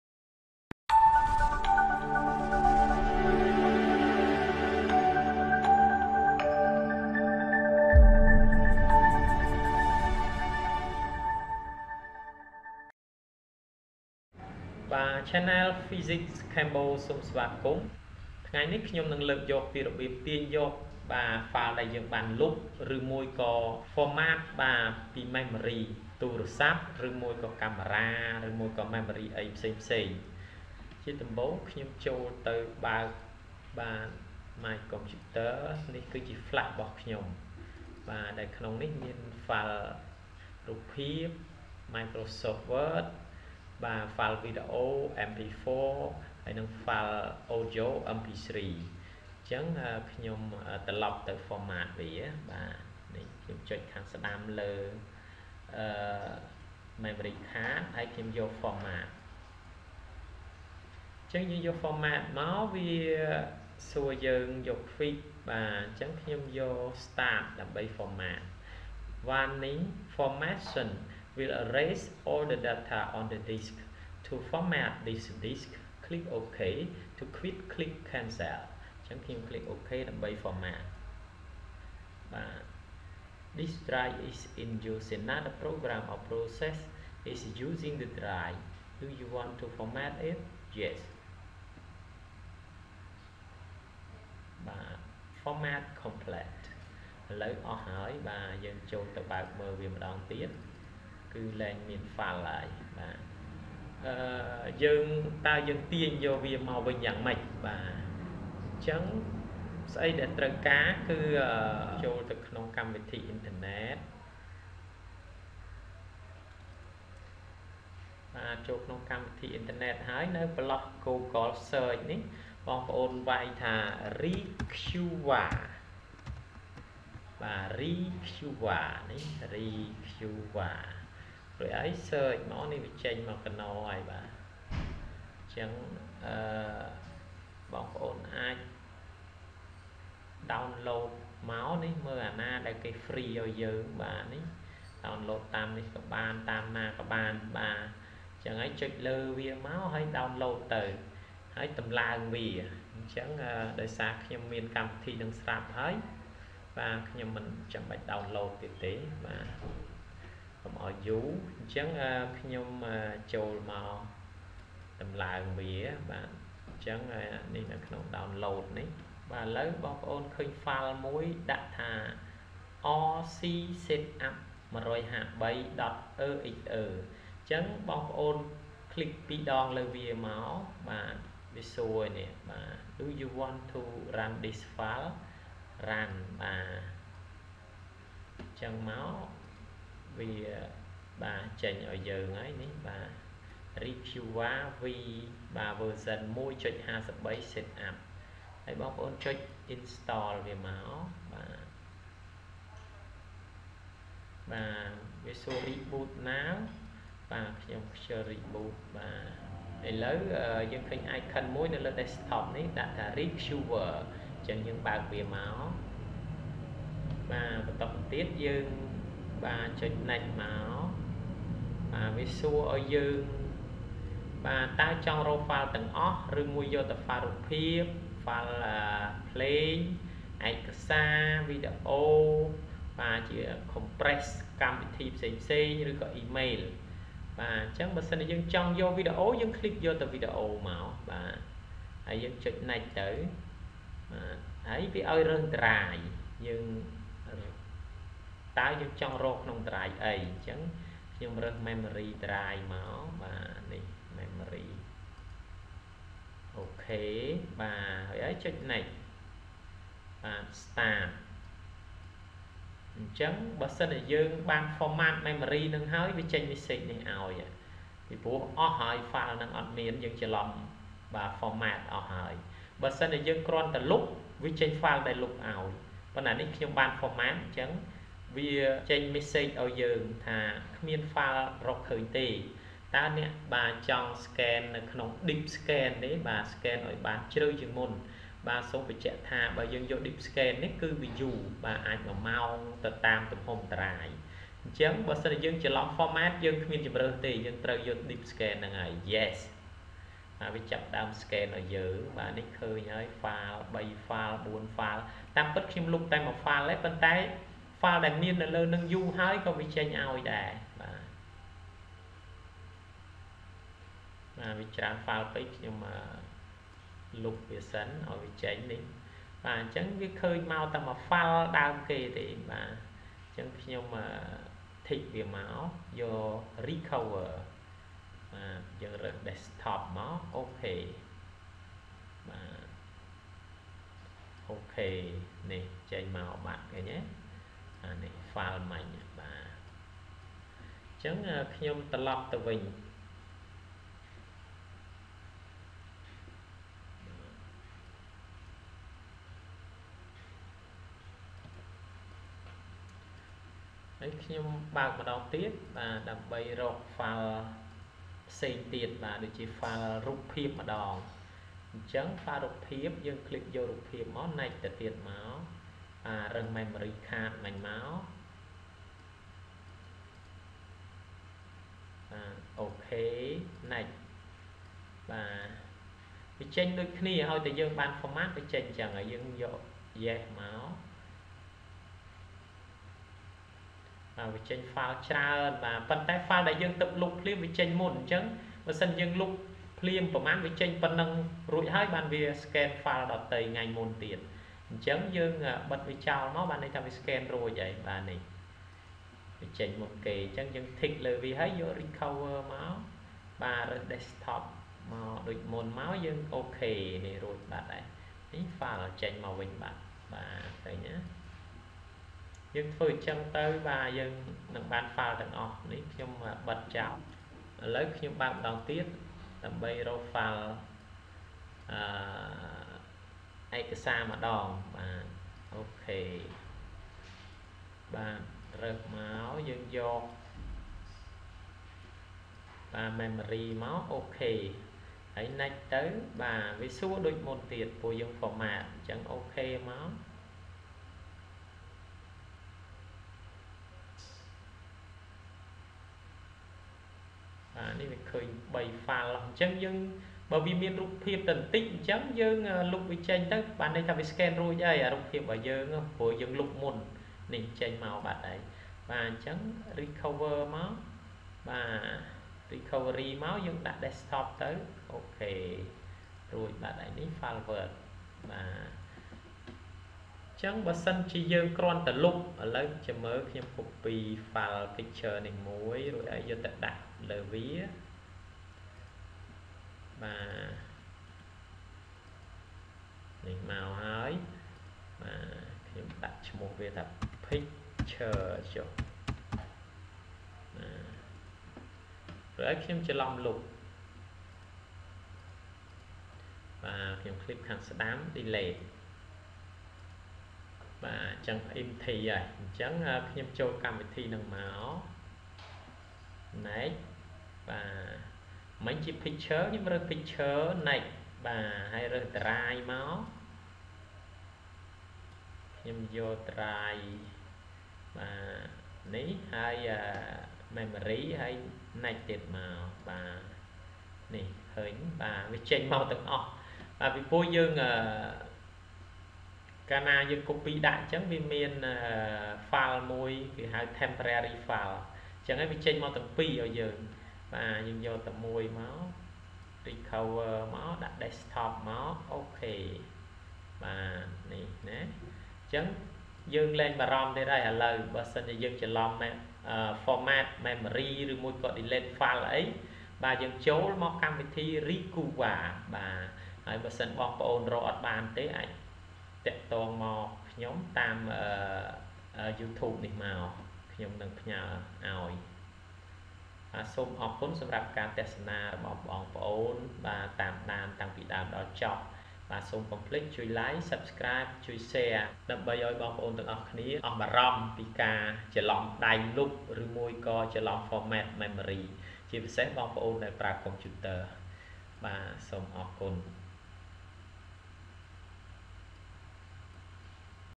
របៀបទាញយកឯកសារដែលបានលុប,ហ្វ័រម៉ាតពីហាតឌិស,មេម៉ូរីខាត|# Retrieve files from deleted,formatted drives
How to Retrieve files from deleted files or a formatted drive.